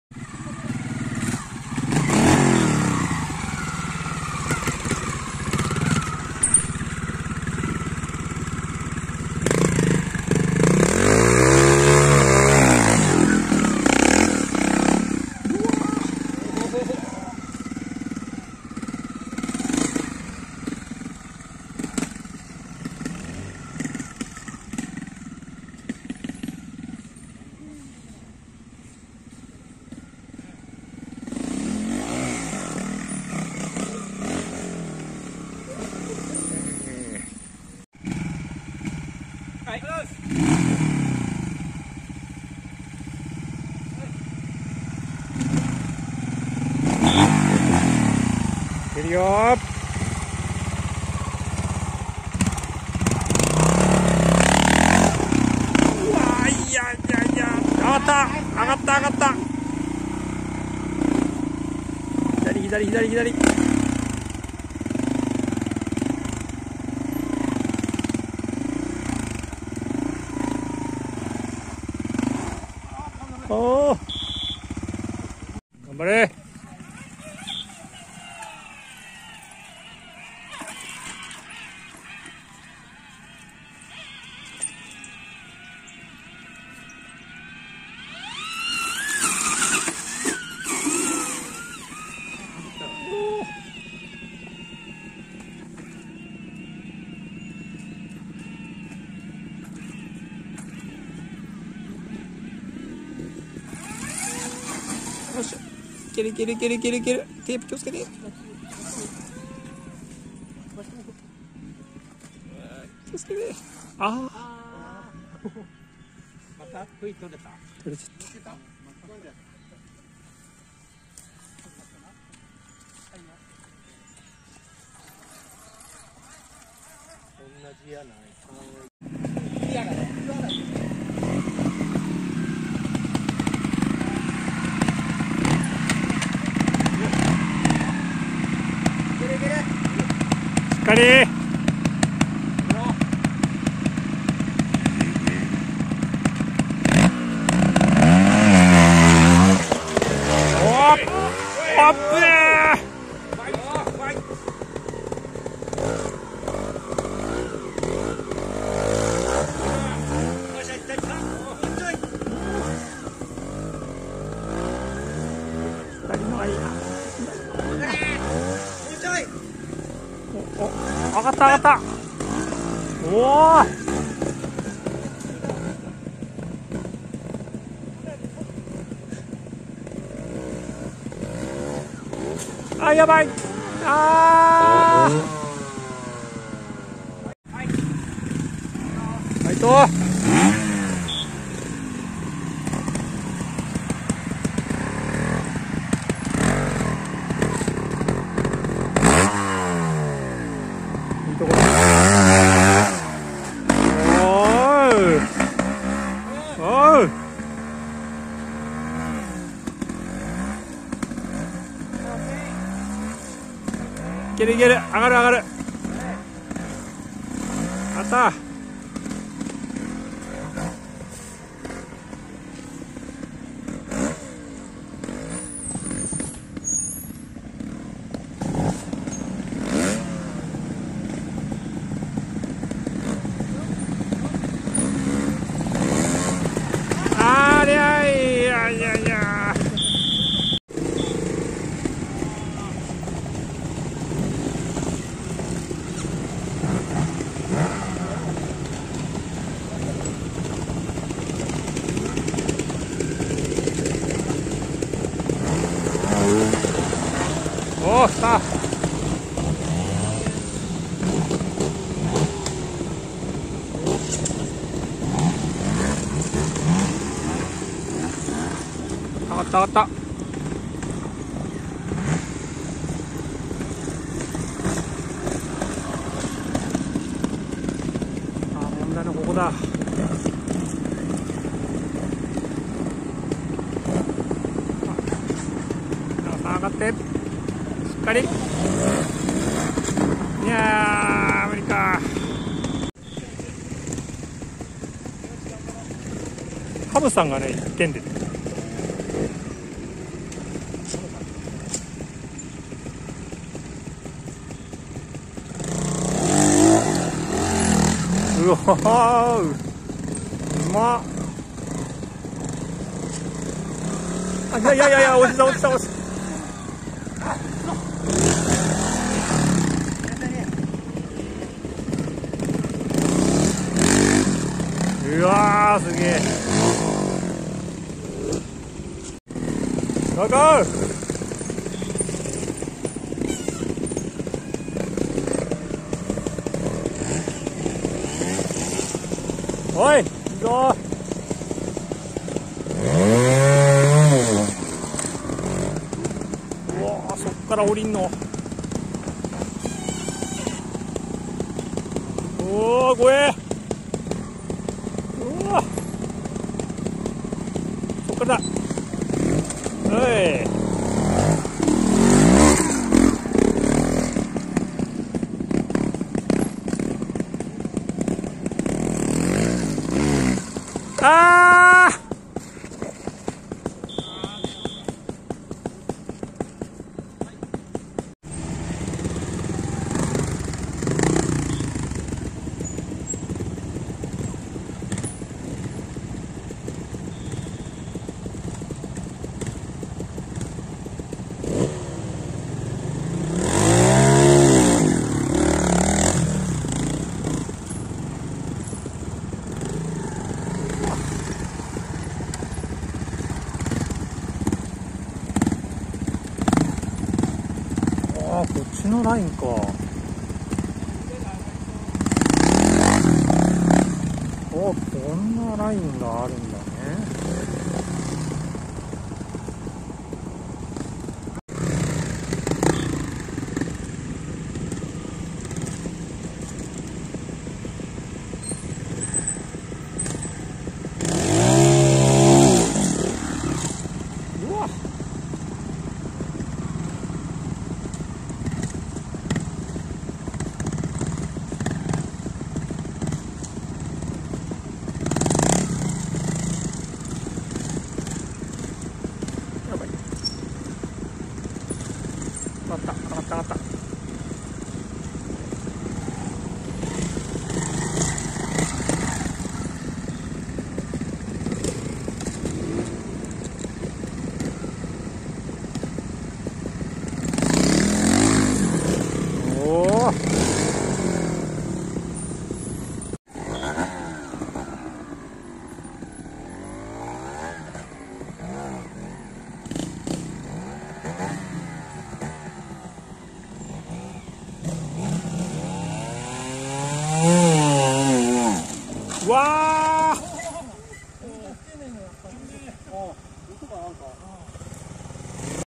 【国際B級＆国際A級 セクション９】2024/06/23 関東トライアル選手権シリーズ第６戦 埼玉大会（オフロードパーク白井）
2024/06/23にオフロードパーク白井（茨城県桜川市）で行われた、"関東トライアル選手権　埼玉大会"の様子をご覧ください。